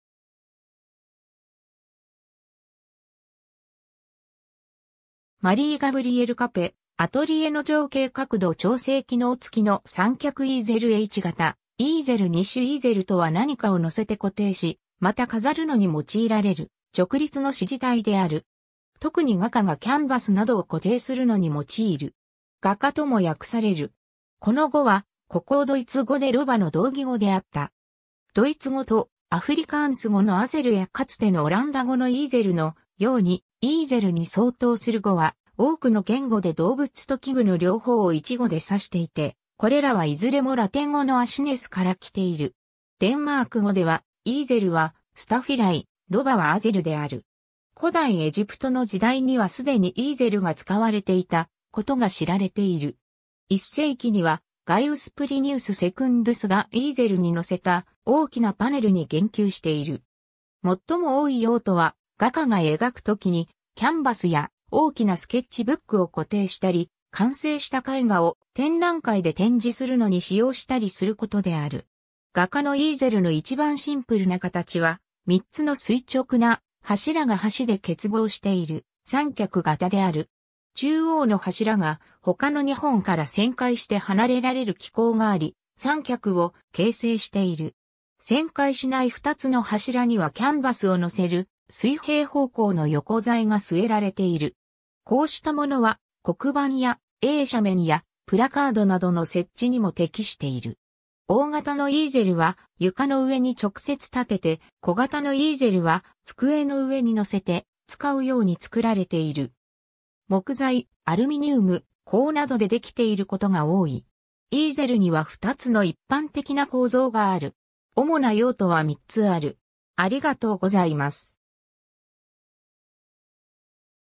イーゼル
マリー＝ガブリエル・カペ『アトリエの情景』(1808) 角度調整機能つきの三脚イーゼル H型イーゼル2種 イーゼル（英語: easel）とは何かを載せて固定し、また飾るのに用いられる直立の支持体である。
特に画家がキャンバスなどを固定するのに用いる。
「画架」とも訳される。
 この語は古高ドイツ語でロバの同義語であった。
ドイツ語とアフリカーンス語のeselやかつてのオランダ語のezel（画架のイーゼルはschildersezel「画家のロバ」と書かれるのが一般的）のように、イーゼルに相当する語は多くの言語で動物と器具の両方を1語で指していて、これらはいずれもラテン語のAsinus（英語のロバassの語源でもある）から来ている。
デンマーク語ではイーゼルはstaffeli、ロバはæselである。
 古代エジプトの時代には既にイーゼルが使われていたことが知られている。
1世紀には、ガイウス・プリニウス・セクンドゥスがイーゼルに載せた大きなパネルに言及している。
 最も多い用途は、画家が描く時にキャンバスや大きなスケッチブックを固定したり、完成した絵画を展覧会で展示するのに使用したりすることである。
画家のイーゼルの一番シンプルな形は、3つの垂直な柱が端で結合している「三脚」型である。
中央の柱が他の2本から旋回して離れられる機構があり、三脚を形成している。
旋回しない2つの柱にはキャンバスを載せる水平方向の横材が据えられている。
こうしたものは黒板や映写面やプラカードなどの設置にも適している。
大型のイーゼルは床の上に直接立てて、小型のイーゼルは机の上に載せて使うように作られている。
木材・アルミニウム・鋼などで出来ていることが多い。
 イーゼルには2つの一般的な構造がある。
 主な用途は3つある。